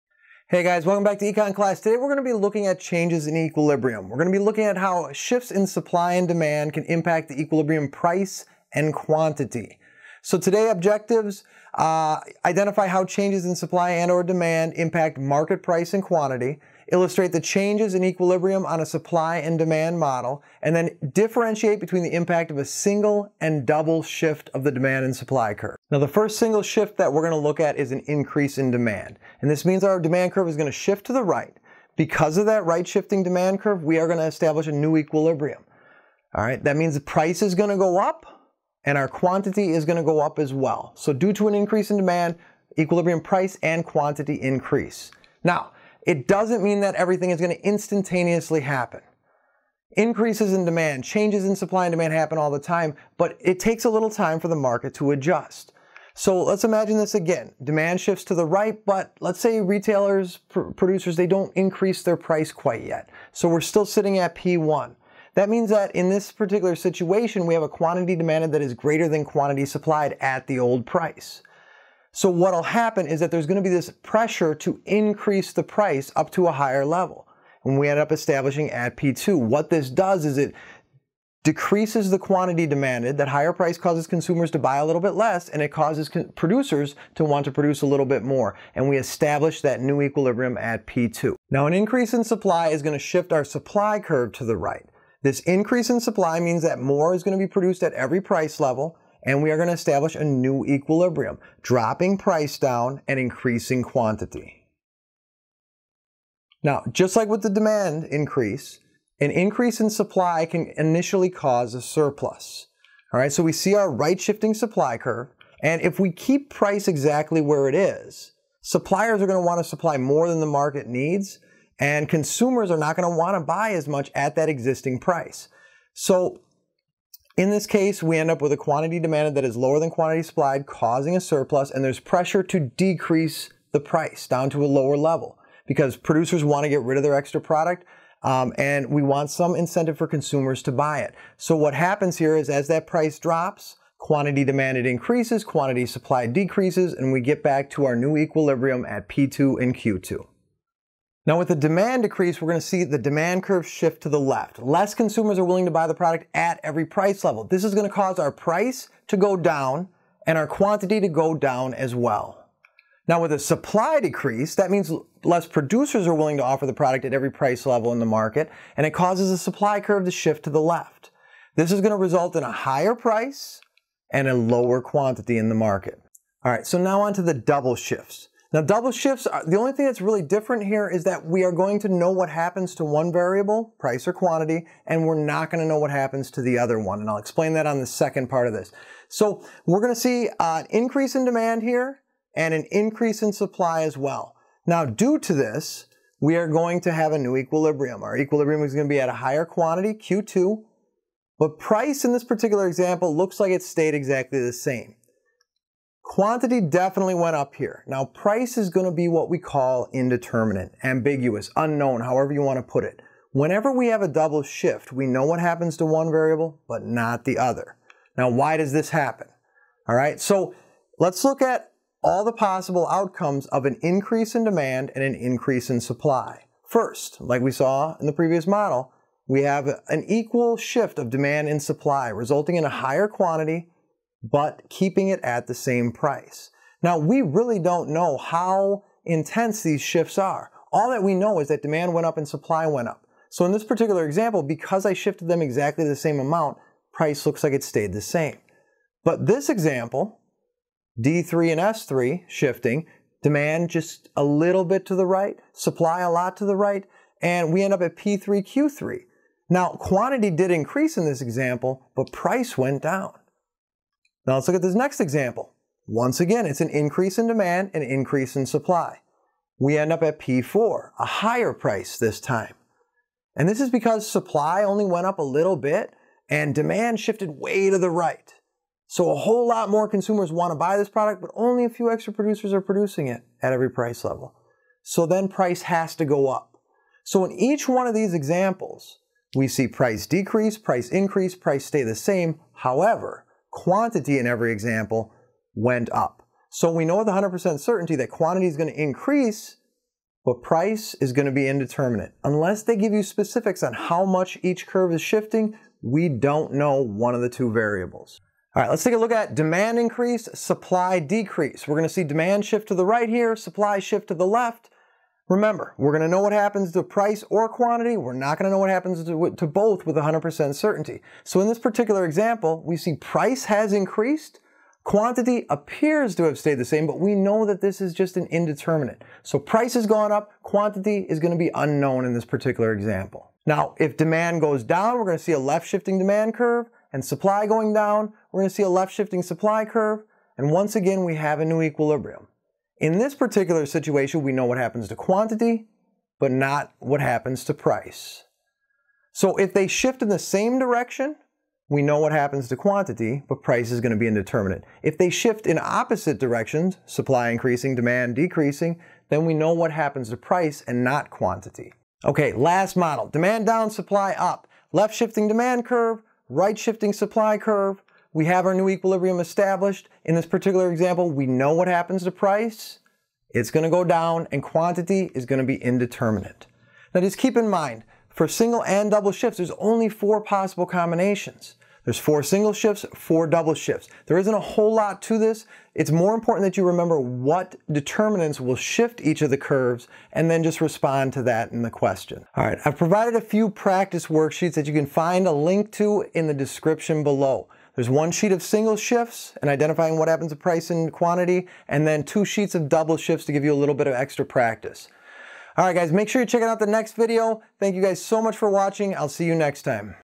Changes in Equilibrium - Single and Double Shifts - Microeconomics 2.05 - Unit 2 - Supply & Demand
AP Micro 2.05 - Unit 2 - Supply and Demand -  Lesson 5 - Changes in Equilibrium - Single and Double Shifts